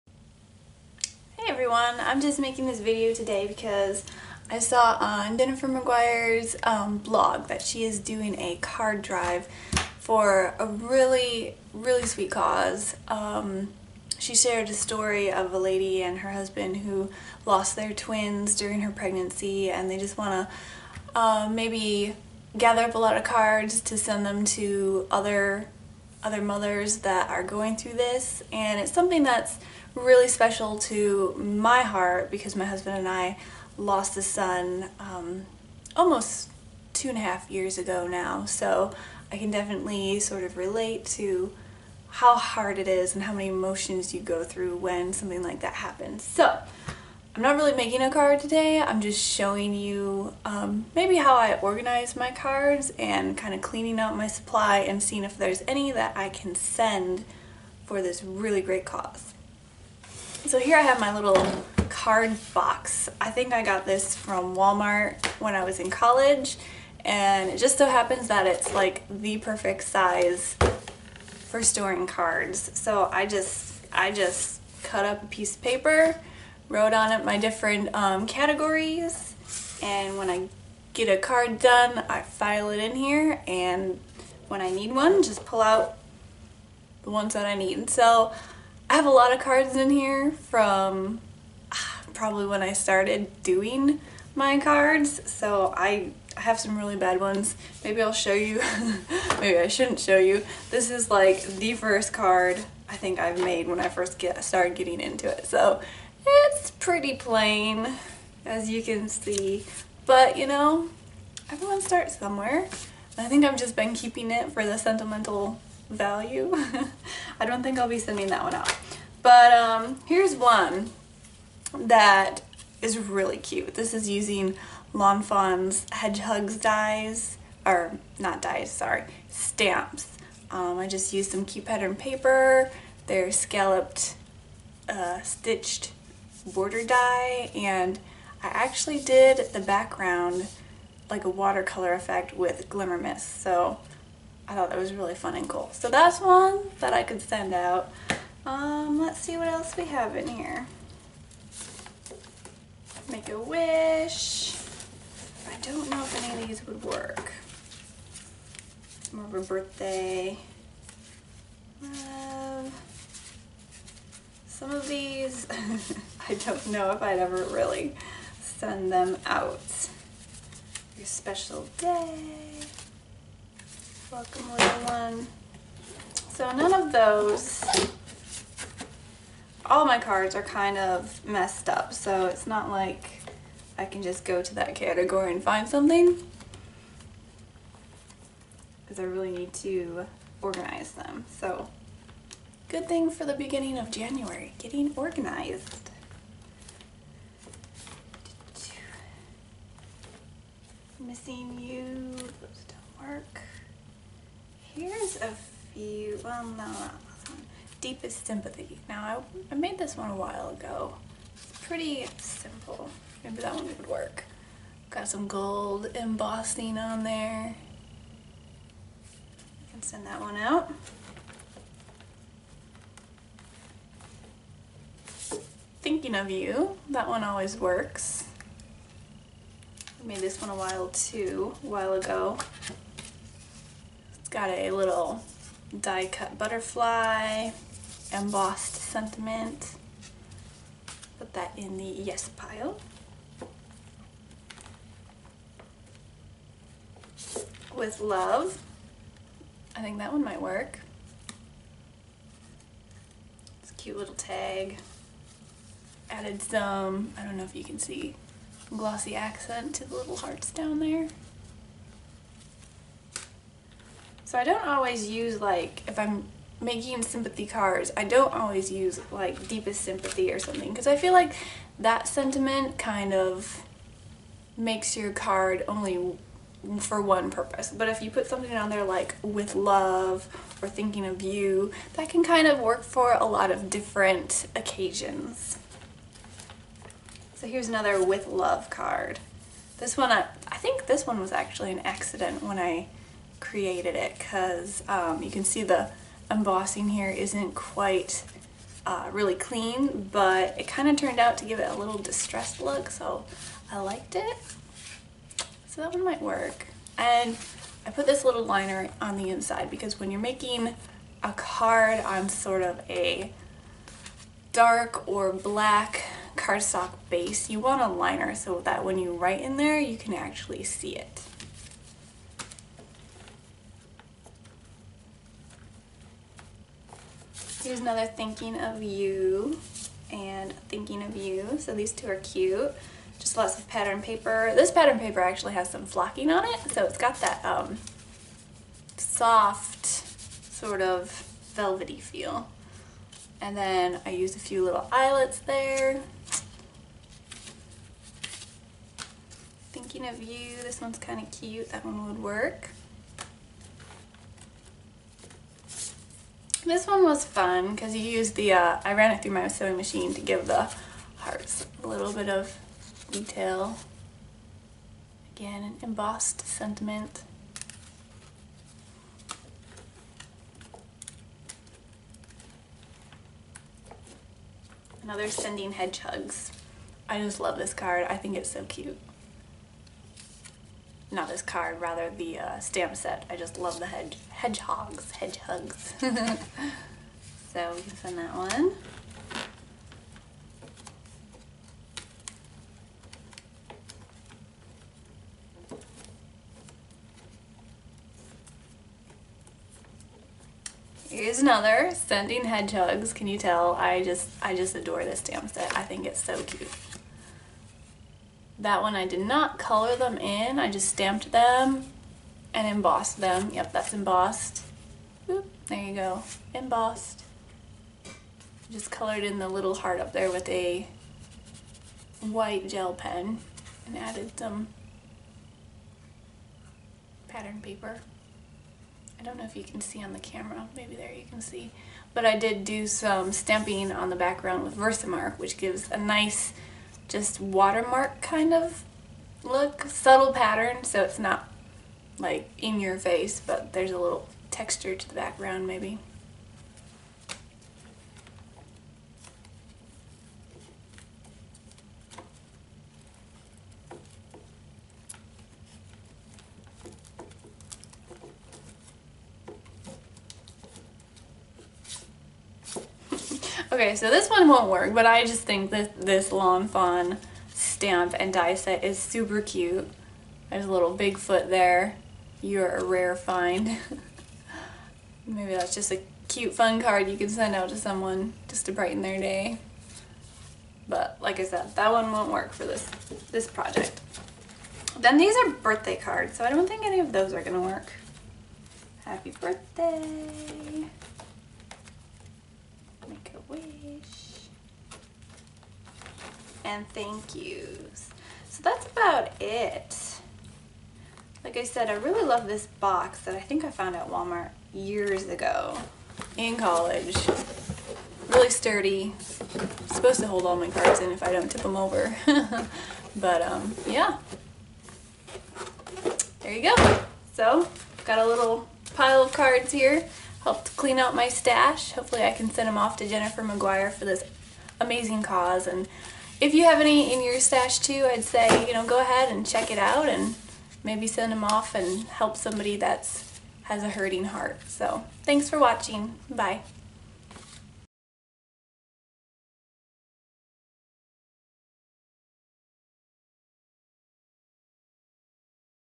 Lasting Hearts Card Drive
Sharing a few examples of cards Im sending off for
the Lasting Hearts Card Drive.